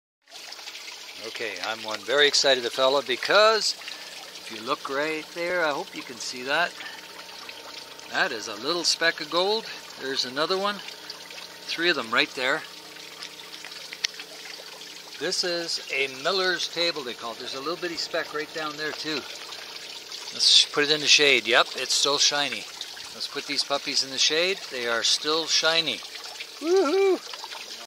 Gold Flakes found in placer pay dirt using a Millers Table
A homemade miller table a small amount of pay dirt from the claim and we have some small gold flakes on the table!